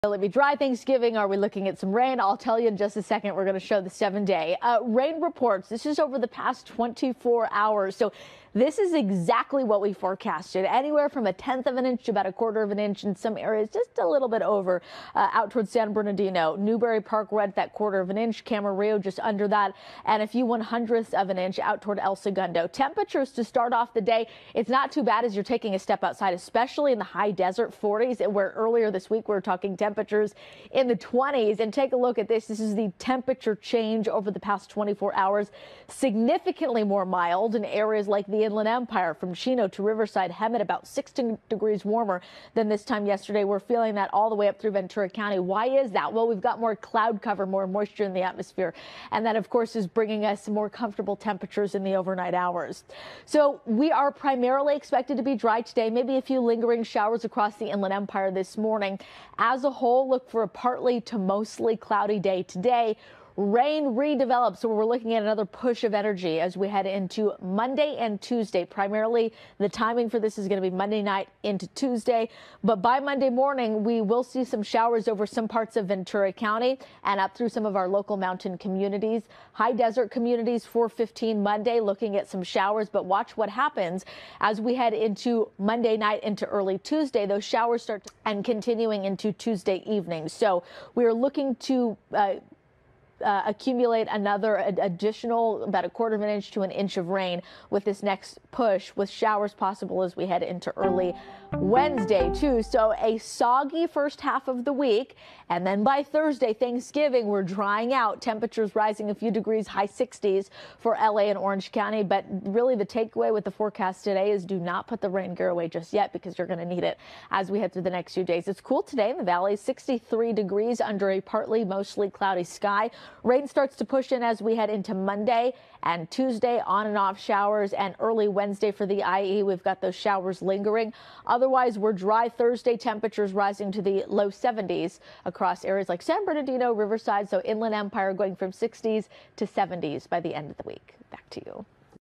Alex Biston 🦊 predicts another storm for southern California
Meteorologist, Alex Biston provides the 7 day forecast predicting rain and cooler temps.